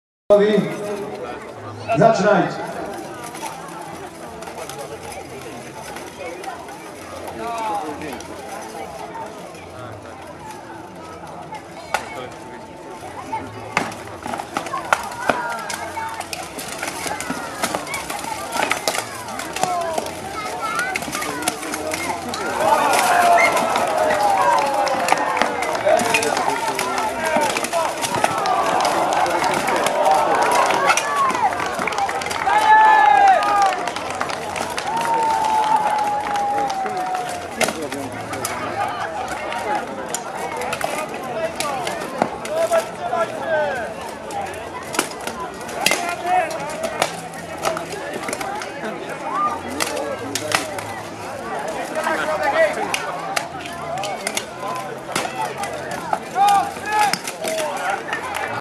Une Fire vs Sułtani Południa 4na4 starcie 2
Walki Drużyn Rycerskich na XVIII Turnieju Rycerskim o Miecz Kasztelana Zamku Ogrodzie­nieck­iego.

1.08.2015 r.